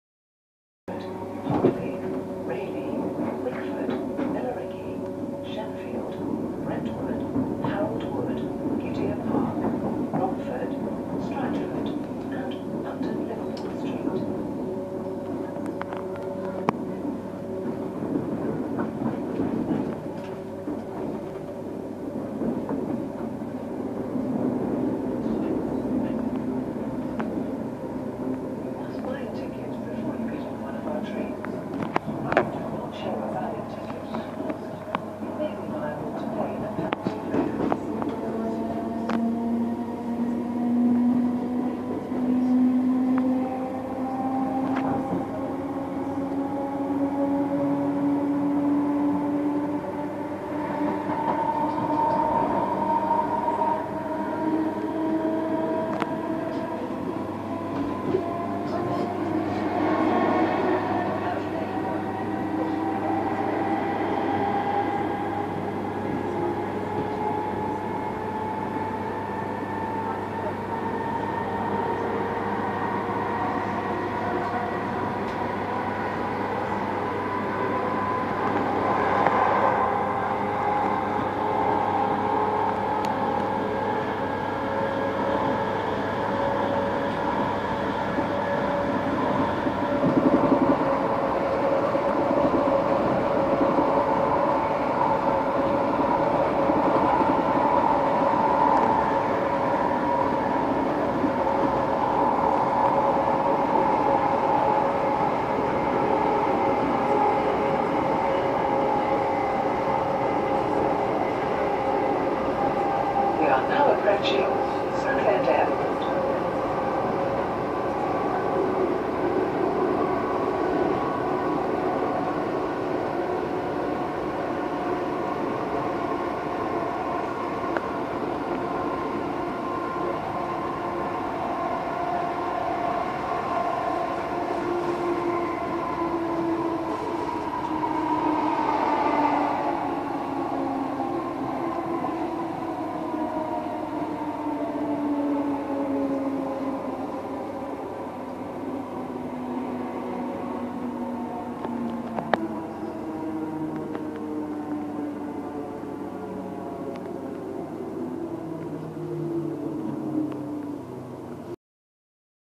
Greater Anglia - Southend Victoria to Southend Airport
We take an Greater Anglia Train from Southend Victoria to Southend Airport.